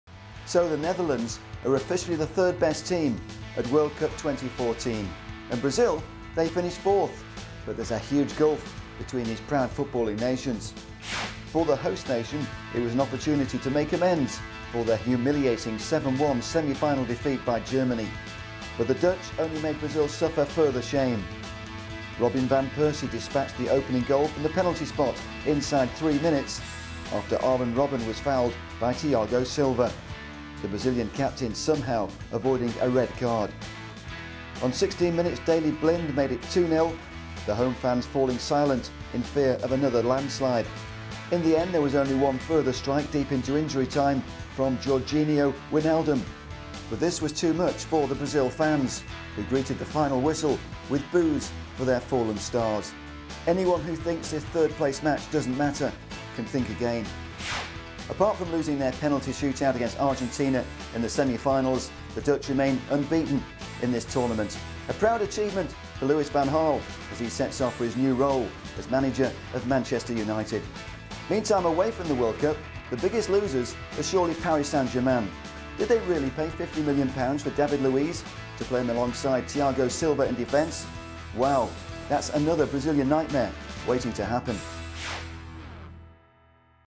Louis Van Gaal's Pride in Holland | Brazil's David Luiz damaged goods
production company: VSI TV for VISIONSPORT

The Netherlands are officially the third best team at World Cup 2014. And Brazil finish fourth. But there's a huge gulf between these proud footballing nations.

 For the host nation it was an opportunity to make amends for their humiliating 7-1 semi final defeat by Germany.

 But the Dutch only made Brazil suffer further shame. Robin Van Persie despatched the opening goal from the penalty spot inside 3 minutes - after Arjen Robben was fouled by thiago silva. The Brazilian captain somehow avoiding a red card.

On 16 mins Daley Blind made it 2-0 .. the home fans falling silent in fear of another landslide.

In the end there was only one further strike deep into injury time from Georginio Wijnaldum

But this was too much for the Brazil fans who greeted the final whistle with boos for their fallen stars. Anyone who thinks this third place match does not matter can think again.

The sight of Dirk Kuyt at the side of the pitch having a cut on his head stapled confirmed just how much the Dutch players wanted to win this game.

 Apart from their penalty shoot out defeat by Argentina in the semi final the Dutch finish unbeaten in this tournament. Louis Van Gaal proud of that achievement as he sets off for his new role as manager of Manchester United.

 Meantime the biggest losers away from the World Cup are surely Paris St Germain. Did they really pay Chelsea 50 million pounds for David Luiz so they can play him in defence alongside Thiago Silva. Wow. Thats another Brazilian nightmare waiting to happen.